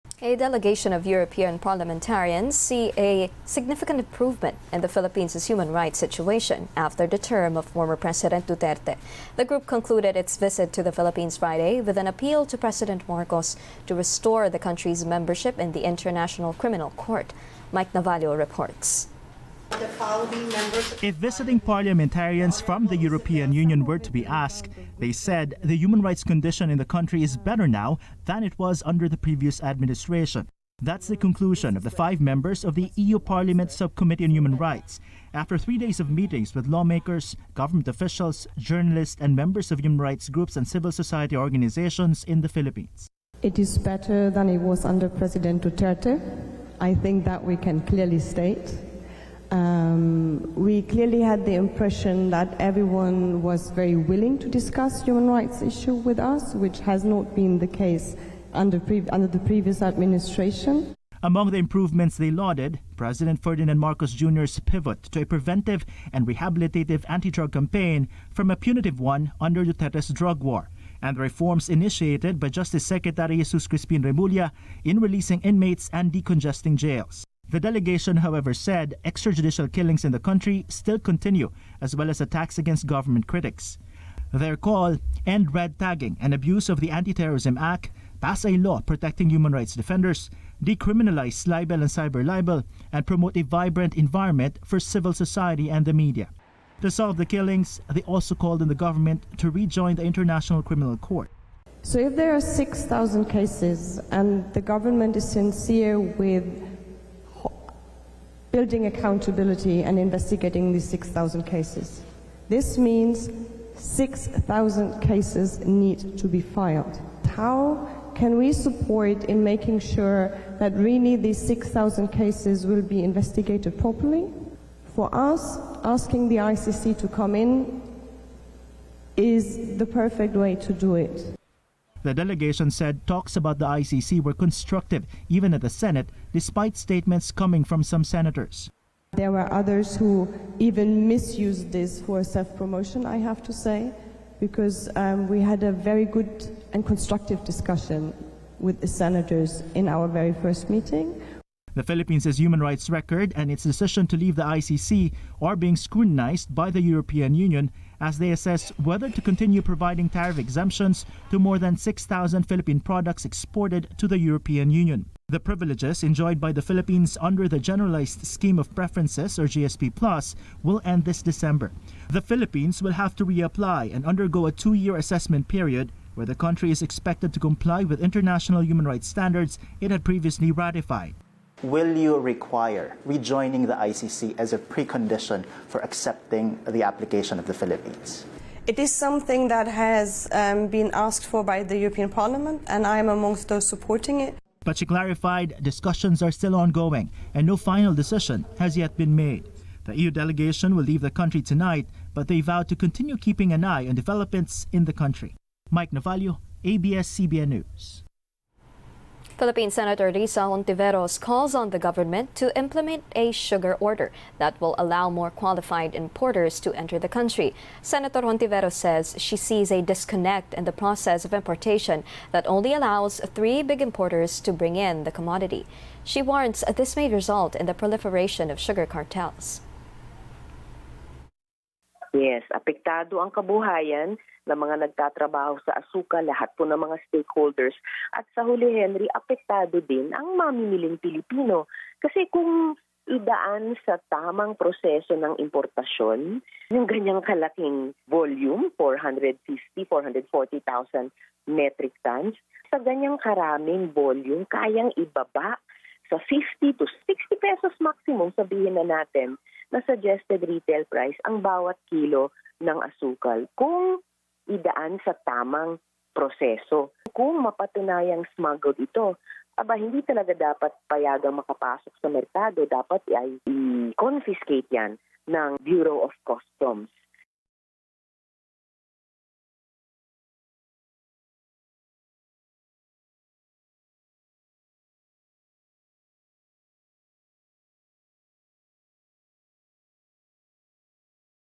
EU Delegation: EJK still an issue, PH urged to rejoin ICC | ANC
Dateline Philippines: A delegation of European parliamentarians sees a significant improvement in the Philippines' human rights situation after the term of former president Duterte. The group concluded its visit to the Philippines Friday with an appeal to President Marcos to restore the country's membership in the International Criminal Court.